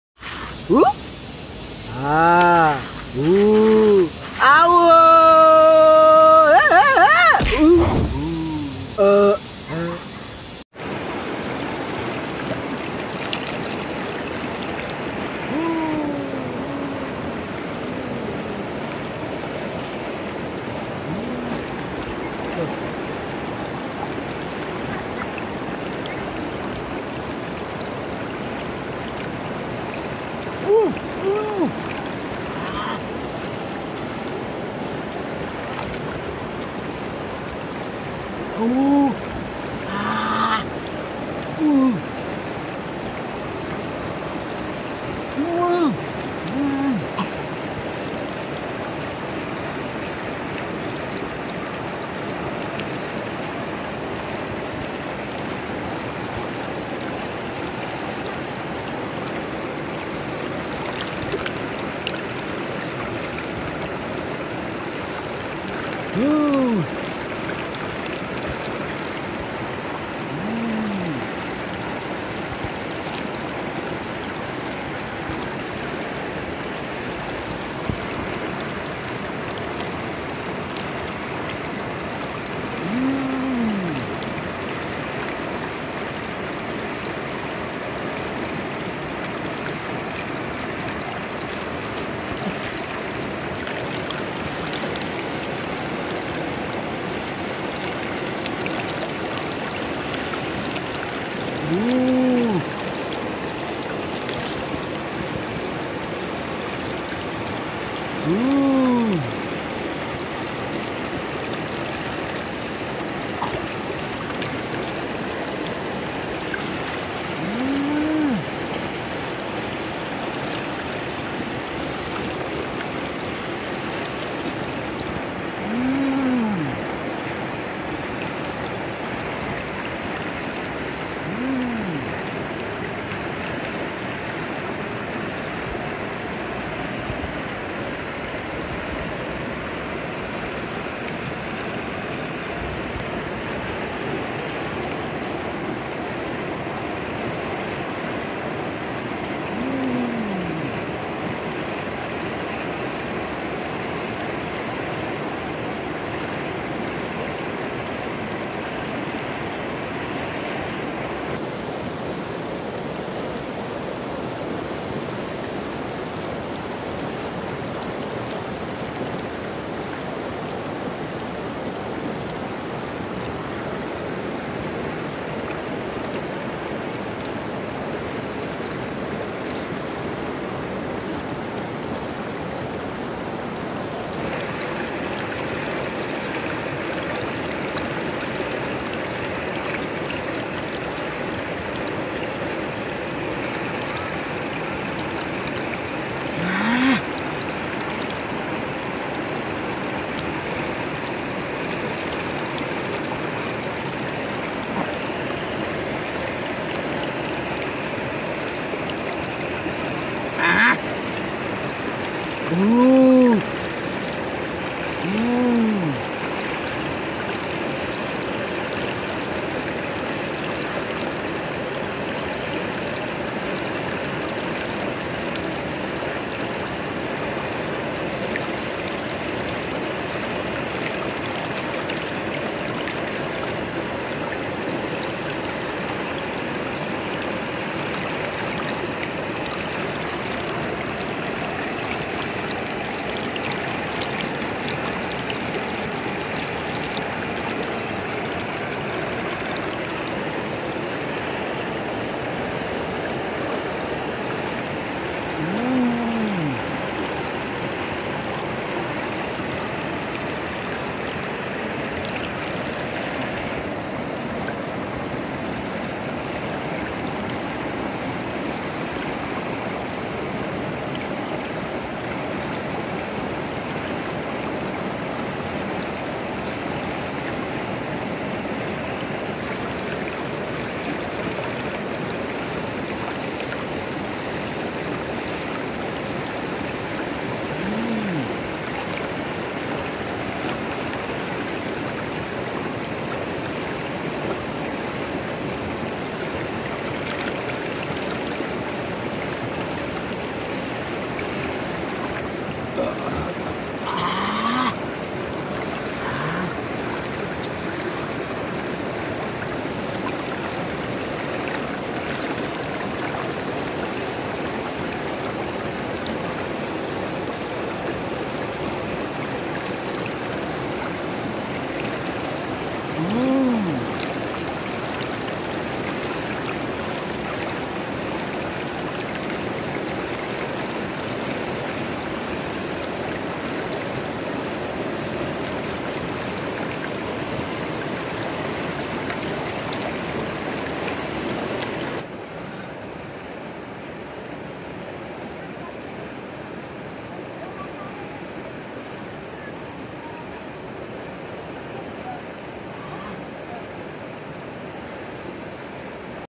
mukbang in river - with a calm vibe
today zan mukbang on the river with beautiful view and calm vibe